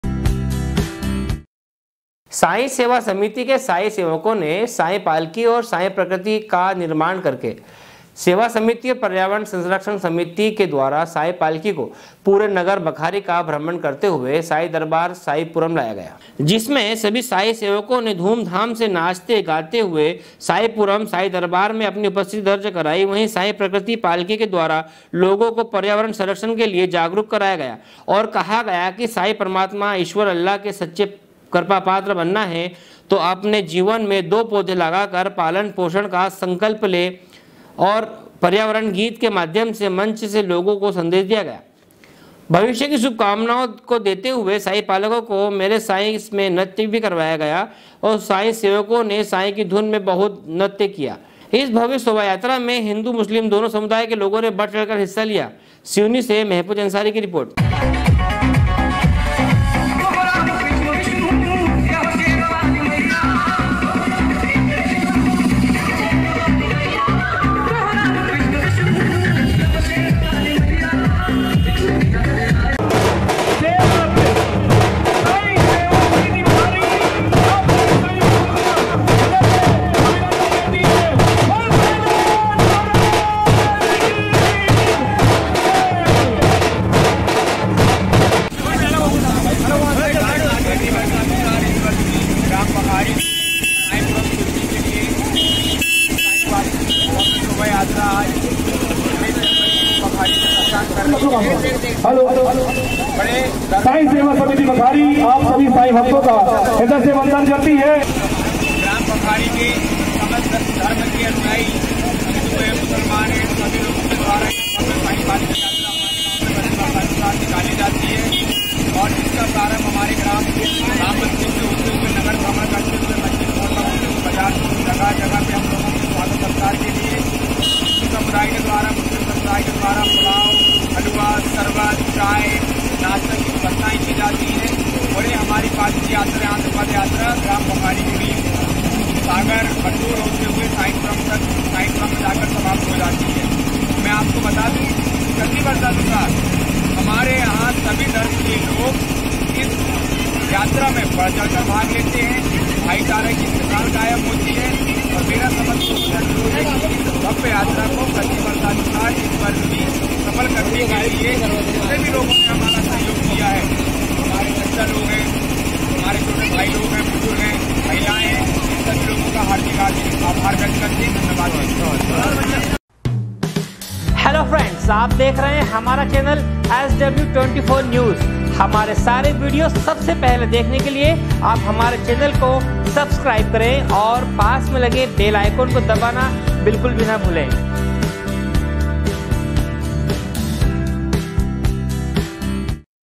SW 24 News Madhyapradesh
साईं सेवा समिति के साईं सेवकों ने साईं पालकी और साईं प्रकृति पालकी का निर्माण करके साईं सेवा समिति और पर्यावरण संरक्षण समिति के द्वारा साईं पालकी को पूरे नगर बखारी का भ्रमण करते हुए साईं दरबार साईं पुरम लाया गया।
सिवनी से महफुज अंसारी की रिपोर्ट